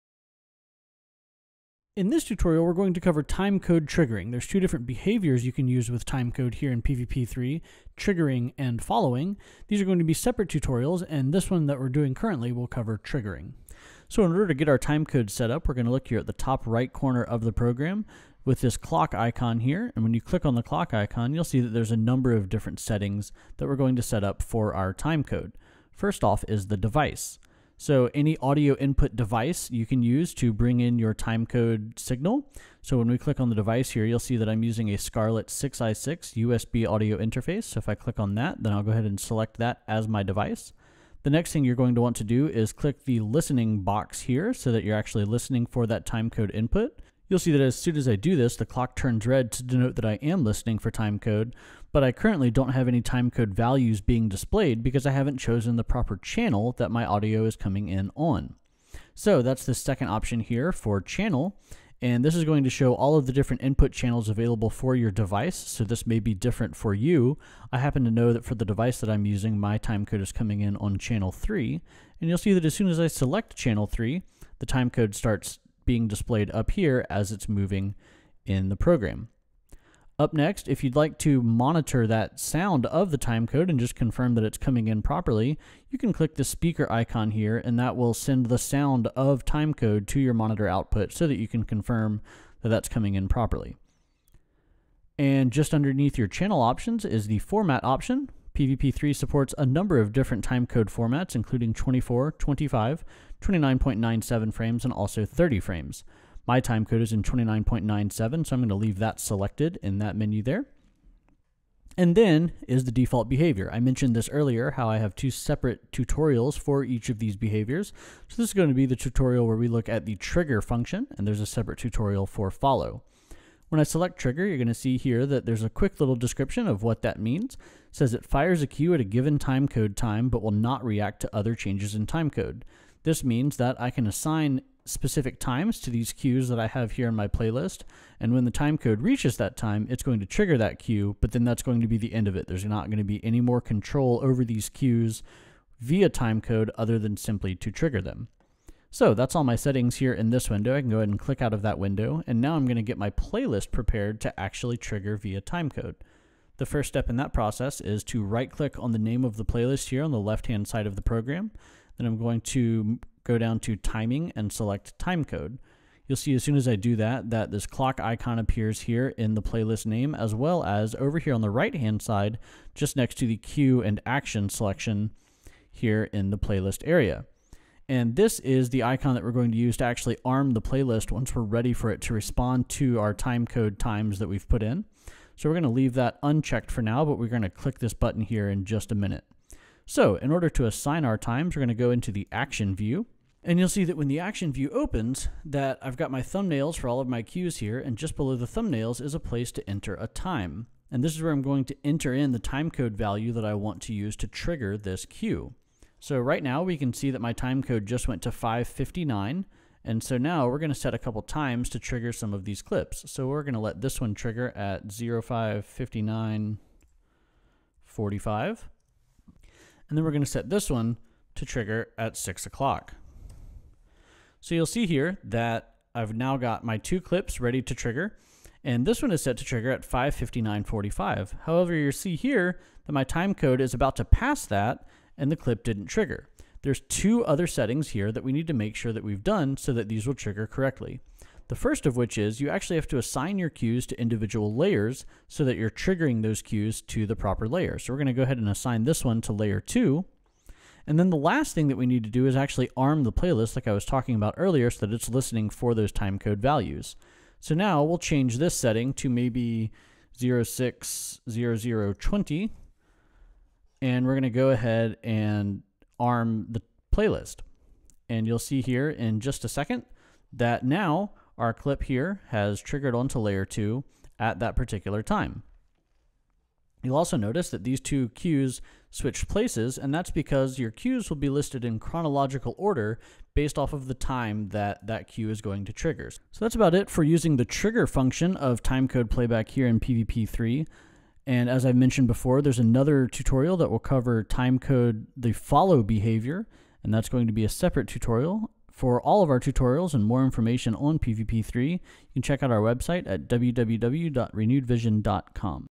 PVP3 | Timecode Triggering
Use timecode to trigger cues to play at a certain time.